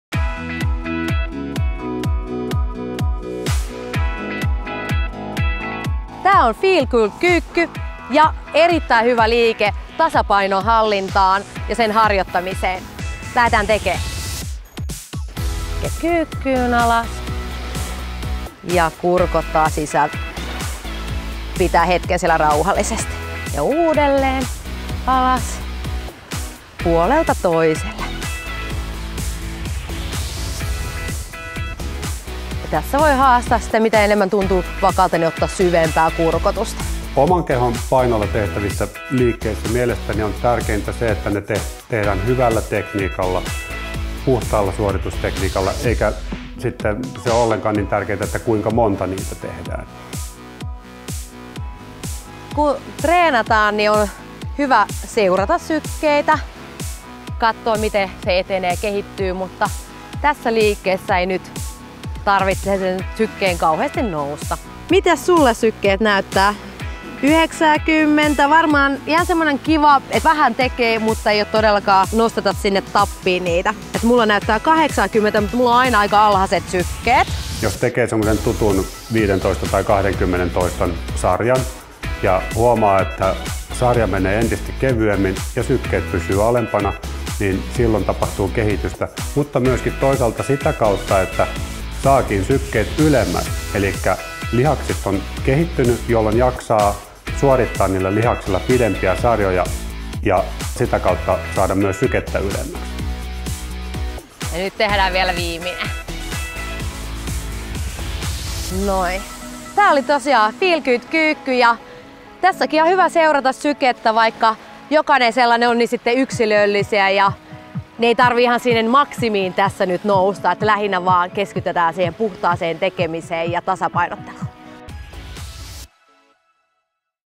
Elixir Fitness: FeelGood-kyykky
Tässä fitness-kyykky-variaatiossa painonsiirto yhdelle jalalle tuo peruskyykkyyn mukaan tasapainotreeniä ja useampia lihasryhmiä. Esiintyjät: Else Lautala, Lauri Kalima ja Hanna Ropo. Asiantuntija: Ville Uronen.